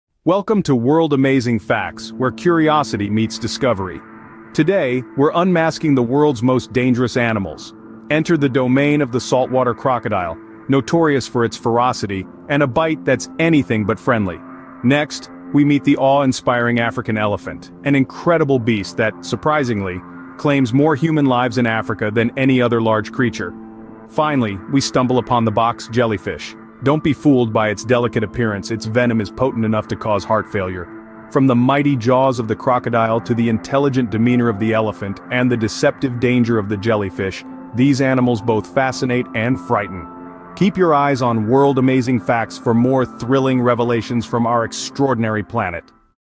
🌍 Explore the World's Most Dangerous Animals on World Amazing Facts🦈🐘🔥
Delve into the world of perilous predators and intriguing creatures on the latest episode of 'World Amazing Facts.' 🌍🐊🐘🔍

In this episode, we uncover the secrets of some of the planet's most fearsome and awe-inspiring animals. From the relentless saltwater crocodile with its bone-crushing bite, to the majestic African elephant, responsible for more human fatalities in Africa than any other large creature, and the deceptively delicate yet deadly box jellyfish with venom capable of causing heart failure.

These formidable beasts command both fascination and intimidation. Join us as we journey from the crocodile's massive jaws to the elephant's remarkable intelligence and the jellyfish's excruciating sting.

Get ready for a captivating exploration of the animal kingdom that's sure to leave you in awe. 🌏📺🦈🐘📡